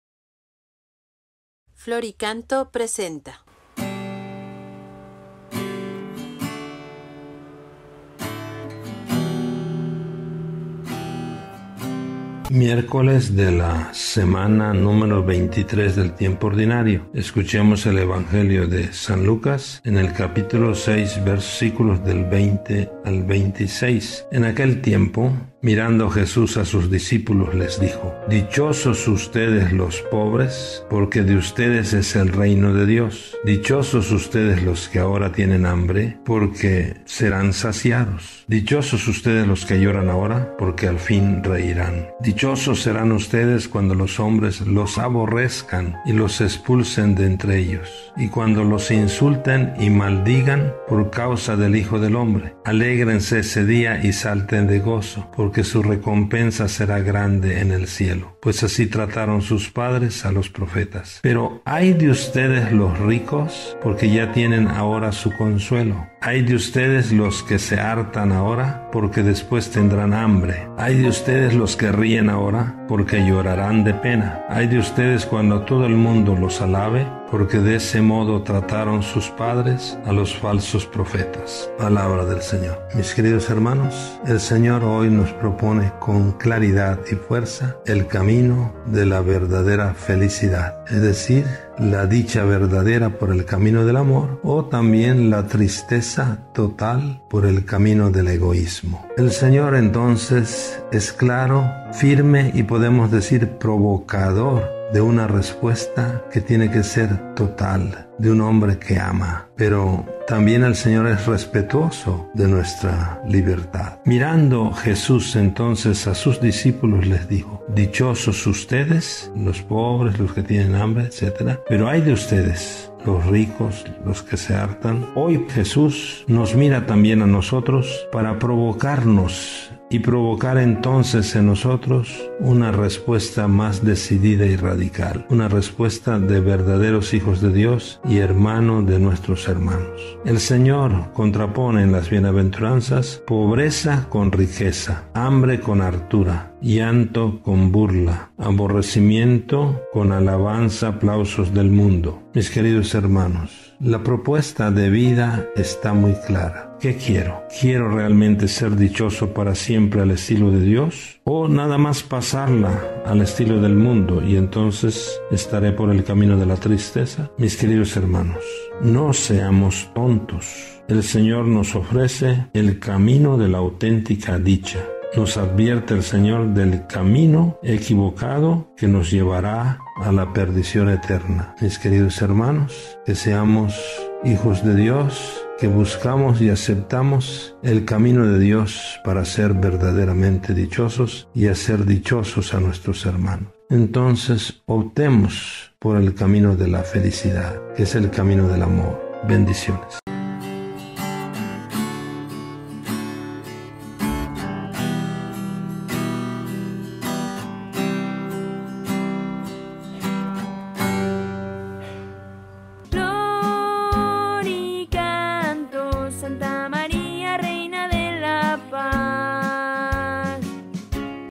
Evangelio del 07 de septiembre de 2022
S. E. Mons. Fabio Martínez Castilla, Arzobispo de Tuxtla Gutierrez nos comparte su reflexión diaria del evangelio y nos invita a hacerlo vida. ¿Qué es lo que más te llama la atención? ¿Qué te invita a hacer hoy?